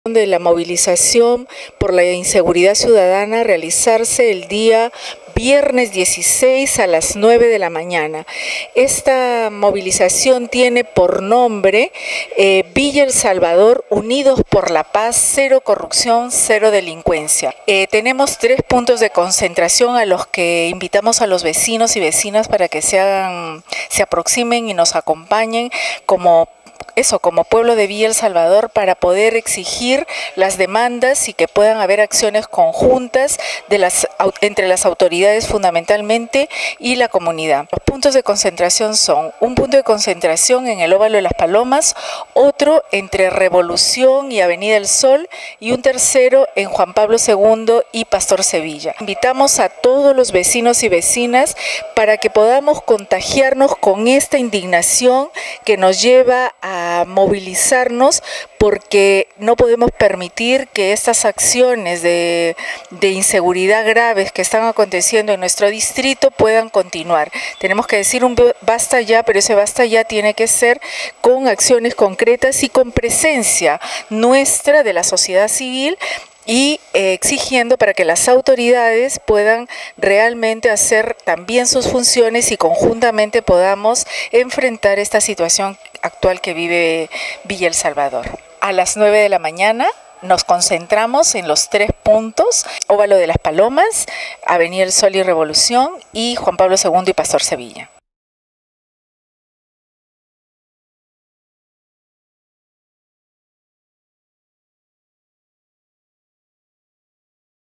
Gran movilización unidos por la paz y la seguridad ciudadana
Viernes 16 de octubre en Villa El Salvador. Puntos de preconcentración 9.00 am Ovalo de las Palomas (Av 200 millas y ruta A) Sector 6: Hospital Juan Pablo II, Primer sector: Av El Sol Con Av. Revolución. Concentración general 10.00 am frontis de la municipalidad de VES. Org. Colectivo por la Seguridad Ciudadana,  Instituciones y Organizaciones sociaciles.